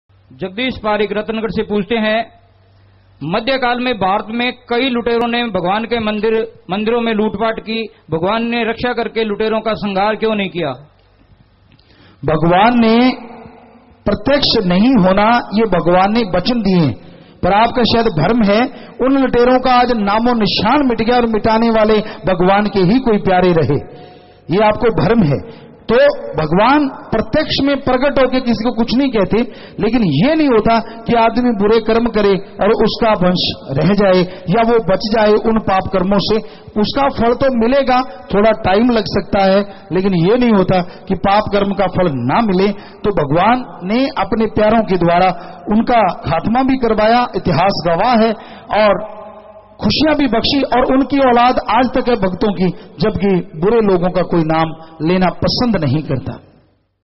Q.558 भारत में कई लोगों में भगवान के मंदिरों में लूटपाट की भगवान ने लुटेरों का संघार क्यों नहीं किया
Q. मध्य काल में भारत में कई लोगों में भगवान के मंदिरों में लूटपाट कि भगवान ने रक्षा कर के लुटेरों का संघार क्यों नहीं किया ?
Saint Dr. Gurmeet Ram Rahim Singh Ji Insan